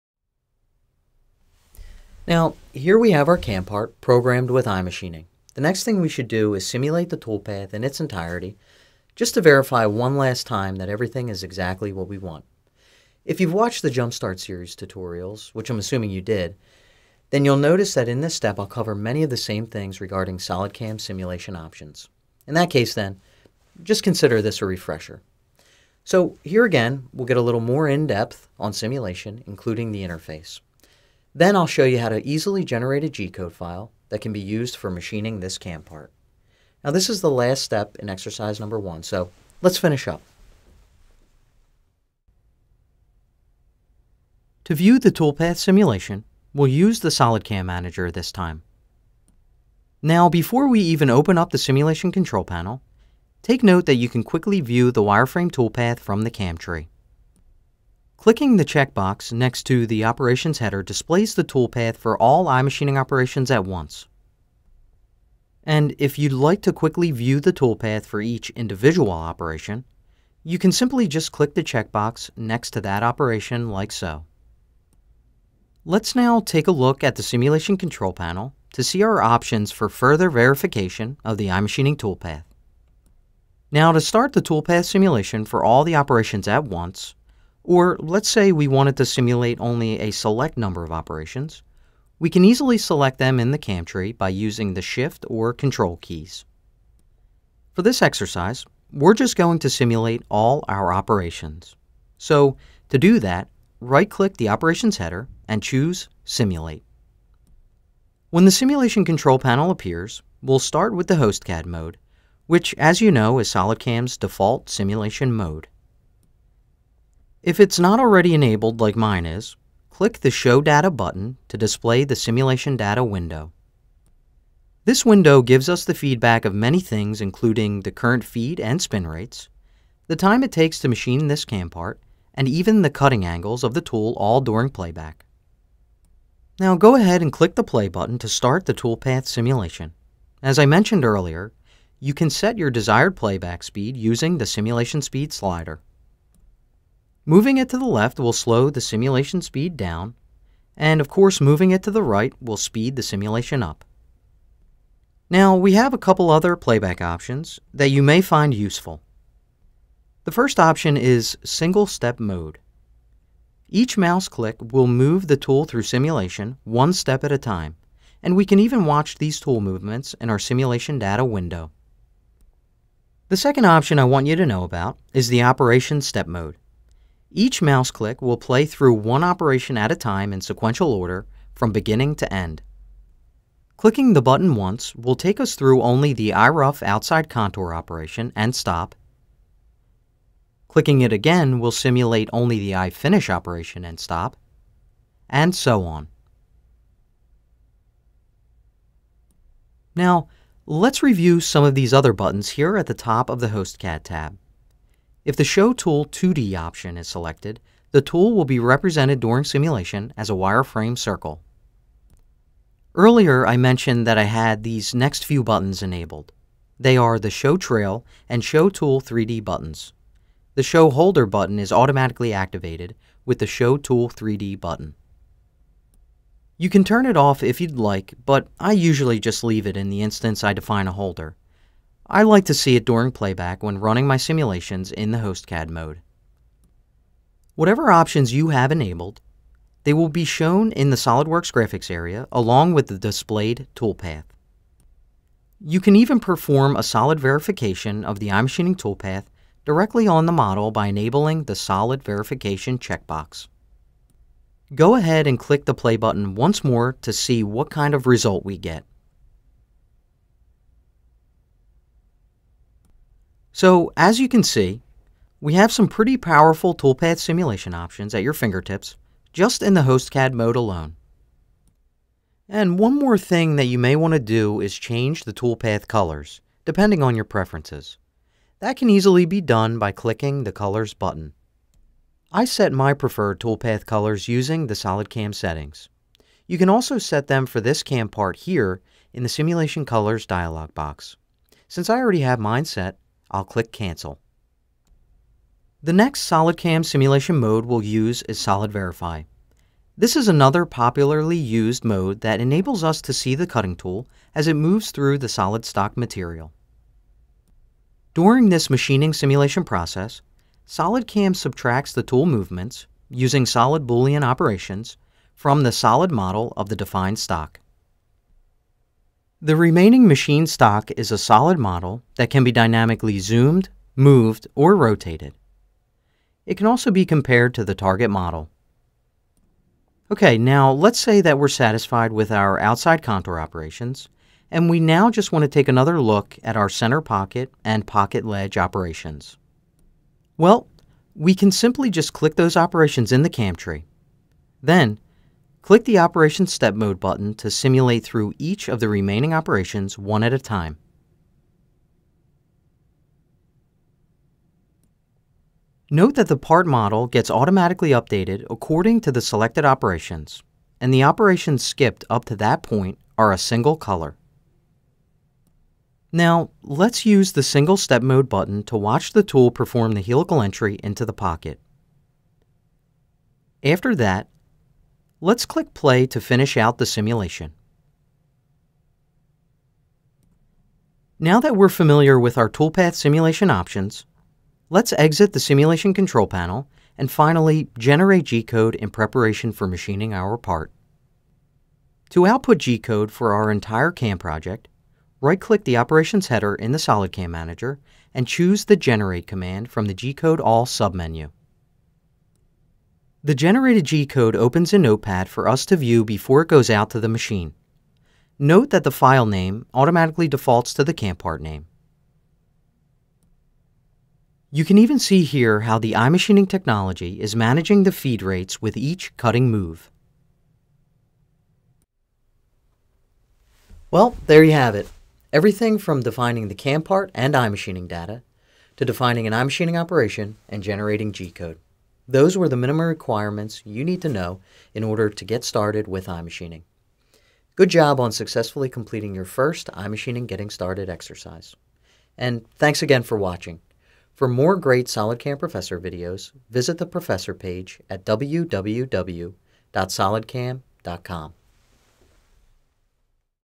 SolidCAM iMachining Getting Started - Walkthrough: Verify the tool path and generate GCode
This is the fifth recording in Exercise #1 of a Getting Started Series called iMachining Walkthrough, where I'll show you how to quickly and easily program a simple part step by step using SolidCAM's iMachining technology. In this video, the iMachining tool path is verified. Included is an in-depth look at SolidCAM's Host CAD simulation options as well as using the SolidVerify simulation mode. A GCode file is also generated and the iMachining technology is shown managing the Feed rates with each cutting move.